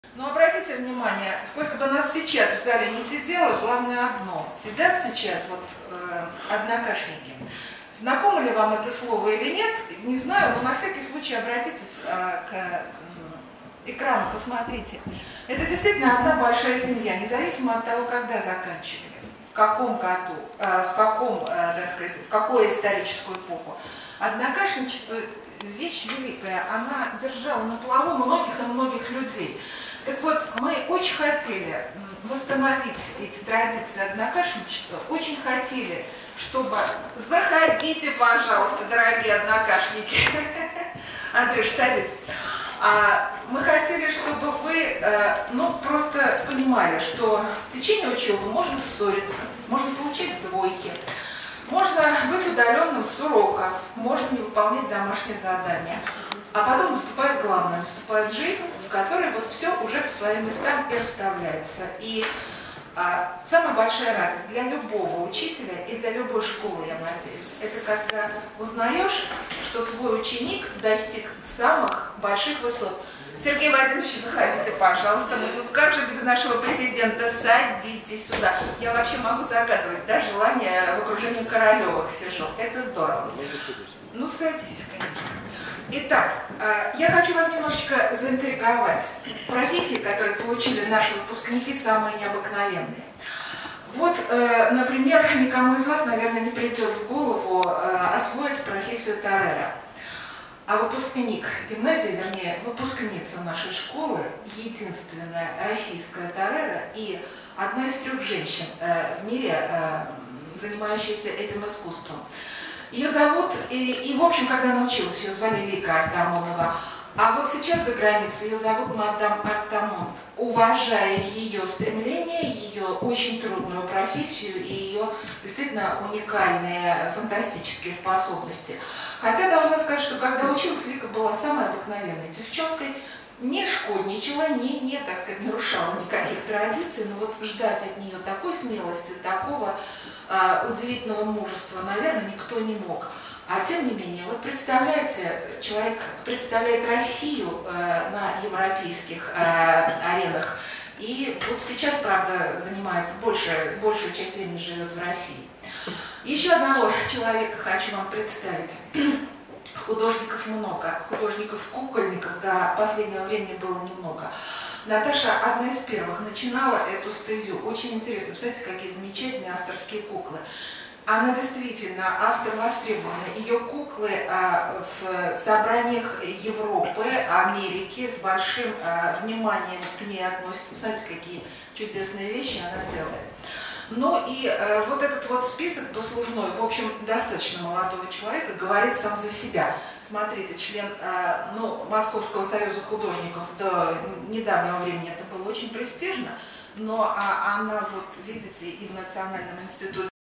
Встреча однокашников Гимназии №1520
Встреча однокашников на празднике в честь открытия мемориальной доски основателю Гимназии №1520 А.С.Капцову, посвященном 120-летию Гимназии.